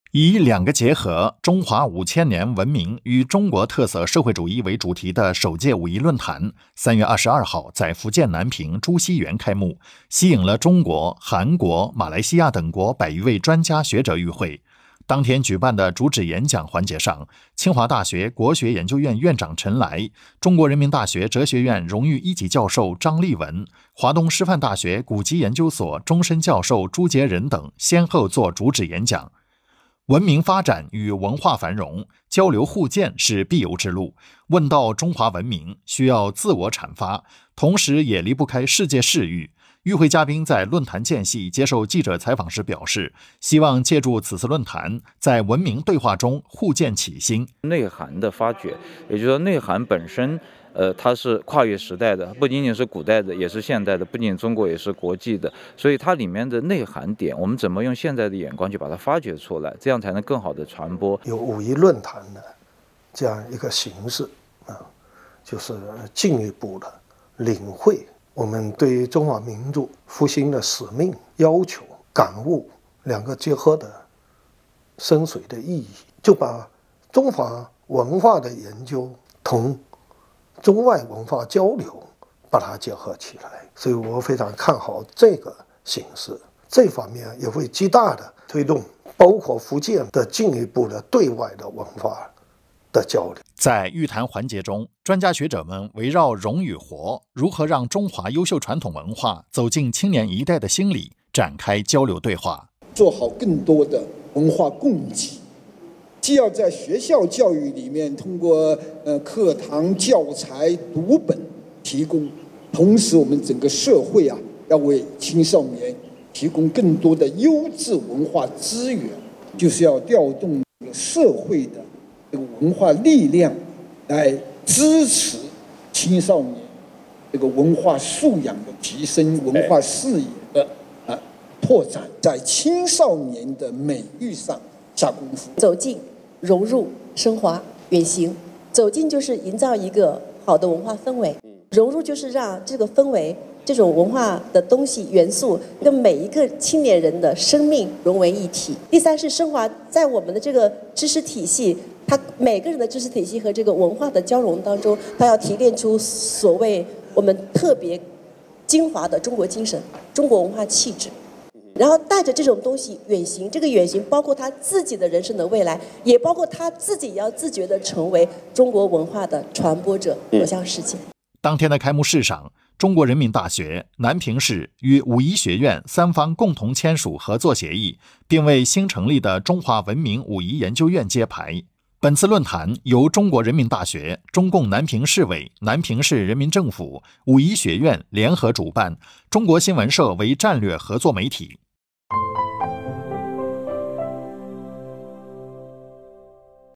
首届武夷论坛召开 中外专家学者畅谈中华文脉
以“‘两个结合’：中华五千年文明与中国特色社会主义”为主题的首届武夷论坛，3月22日在福建南平朱熹园开幕，吸引了中国、韩国、马来西亚等国百余位专家学者与会。

当天举办的主旨演讲环节上，清华大学国学研究院院长陈来，中国人民大学哲学院荣誉一级教授张立文，华东师范大学古籍研究所终身教授朱杰人等先后作主旨演讲。

文明发展与文化繁荣，交流互鉴是必由之路。问道中华文明，需要自我阐发，同时也离不开世界视域。与会嘉宾在论坛间隙接受记者采访时表示，希望借助此次论坛在文明对话中互鉴启新。

在“与谈环节”中，专家学者们围绕《“融”与“活”：如何让中华优秀传统文化走进青年一代的心里》展开交流对话。

当天的开幕式上，中国人民大学、南平市与武夷学院三方共同签署合作协议，并为新成立的中华文明武夷研究院揭牌。

本次论坛由中国人民大学、中共南平市委、南平市人民政府、武夷学院联合主办，中国新闻社为战略合作媒体。（吴晟炜 柯宁）